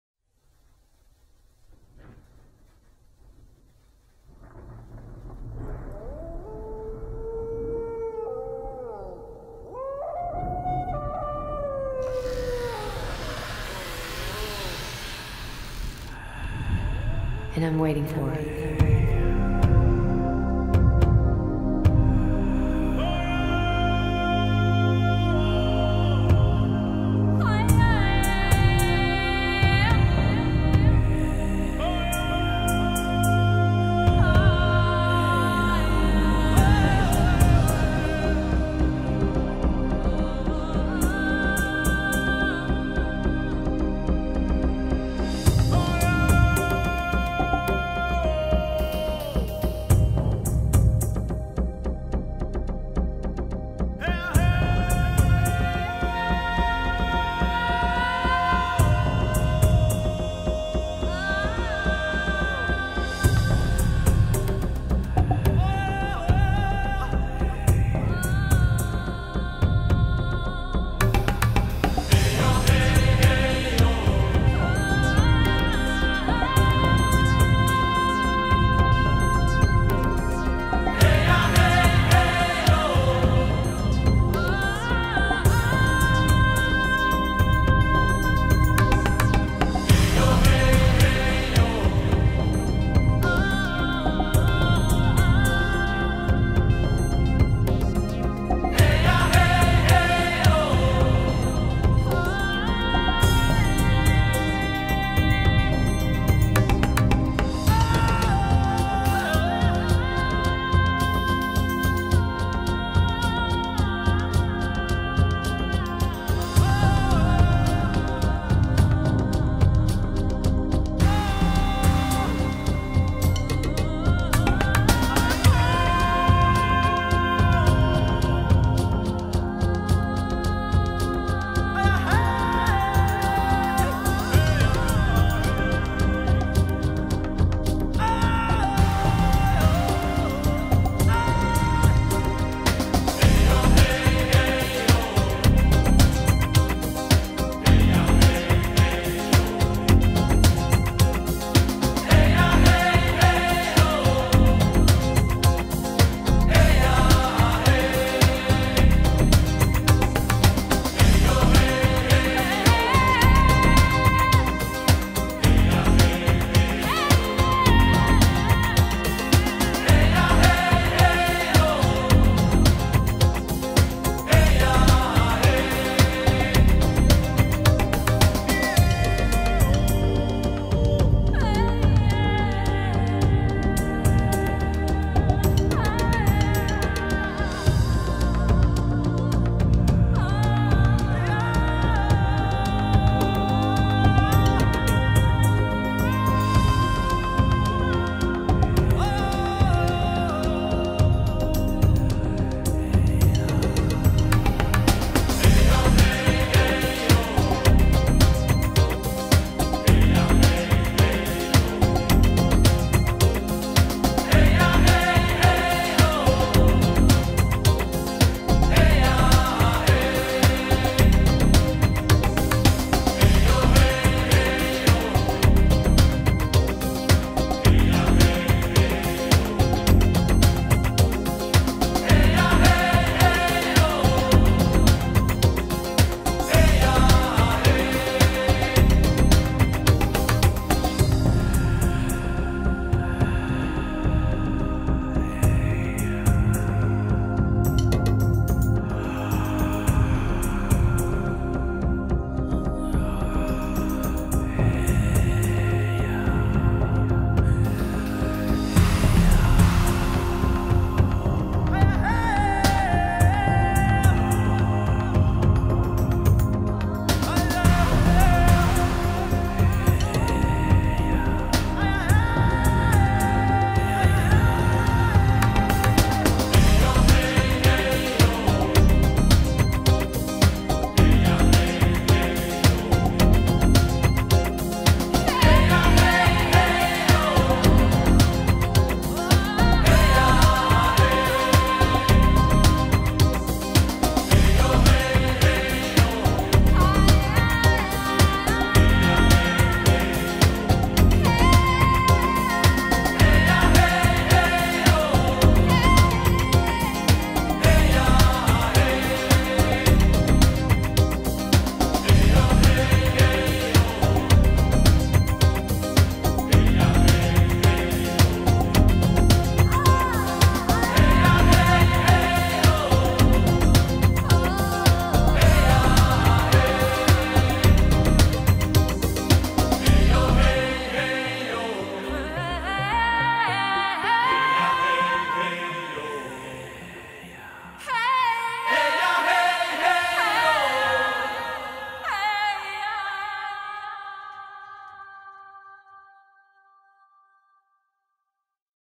Native American - Indian Wisdom (by Stefan Kovačević)
A video representing native american culture made as a tribute.